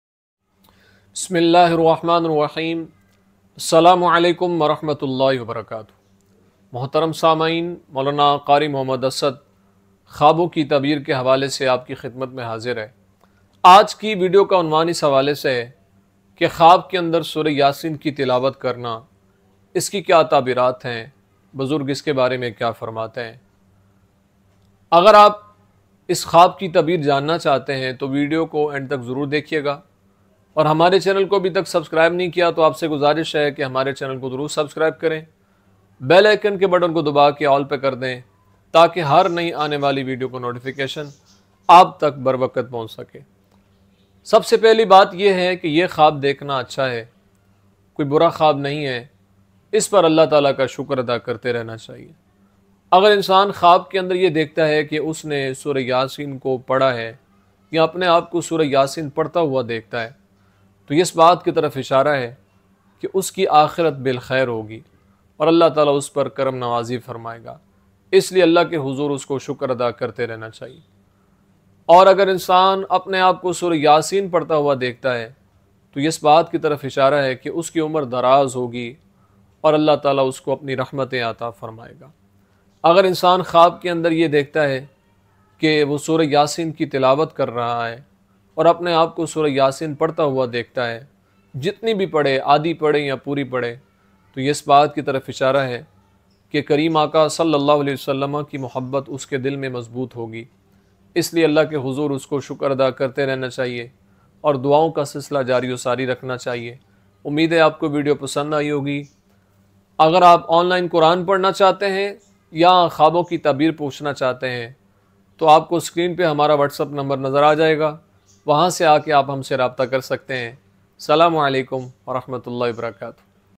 Khwab Mein Surah yaseen Parhna | Khwab Mein Surah yaseen Dekhna | Khwab Ki Tabeer
aj is video mein hum ap sy khwab main sorah yaseen parhne  k mutaliq tabeer share karyn gy . khwab main sorah yaseen parhne   ki mukhtalif tabeerein hain umeed hai k ap sb dekhny walo ko is video sy  kafi faida mily ga or malomat hasil ho gi. end tak zroor dekhiye ga khwab nama yousefi channel will be very helpful and beneficial for both man and women!if you like it so don't forget to subscribe my channel